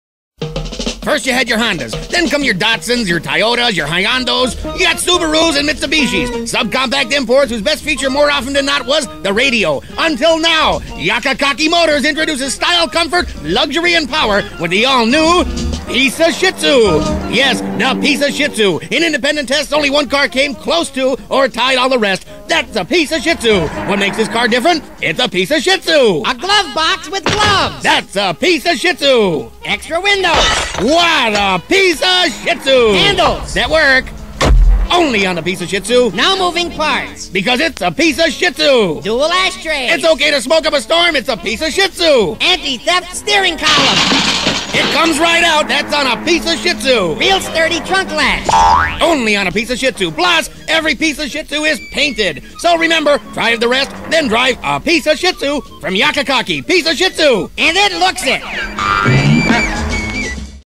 Introducing The All New....Piece-Of-Shit-Su!
WEBSITE ➜ Introducing the all new piece-of-shit-su! by yamomato motors! Get all of the latest and high tech gear in your car by purchasing one through yamomato motors today! located on the highway! They value their customer greatly! visit them